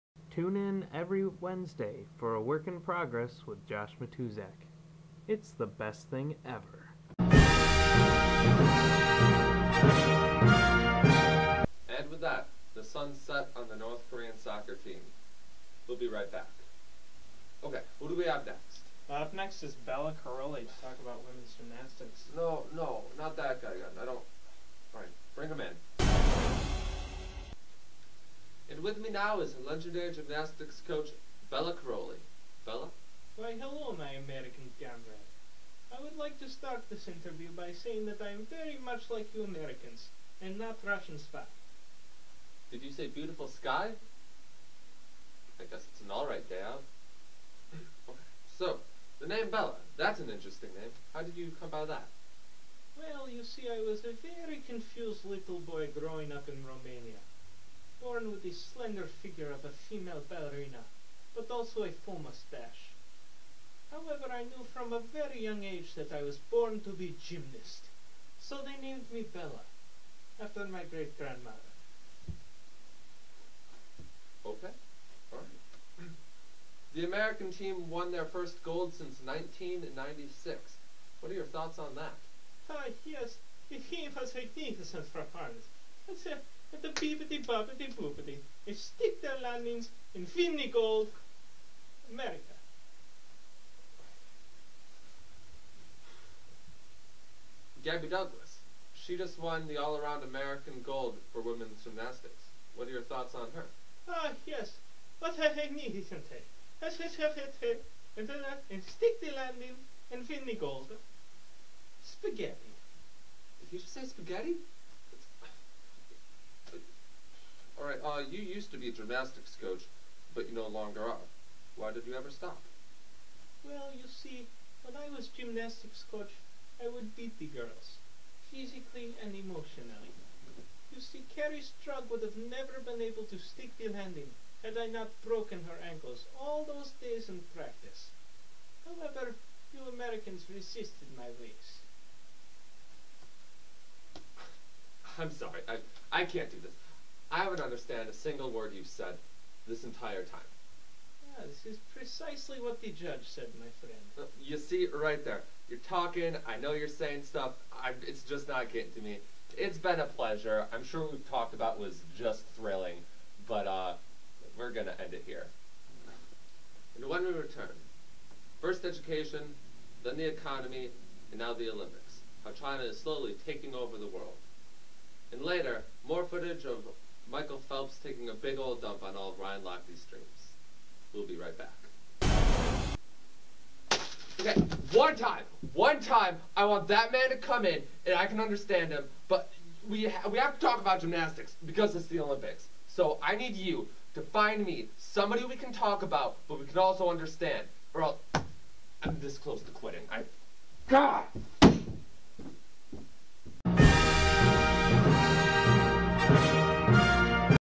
CTV 3- Olympic Special
In this web exclusive, a sports analyst sits down to talk women's gymnastics with the legendary Bela Karolyi.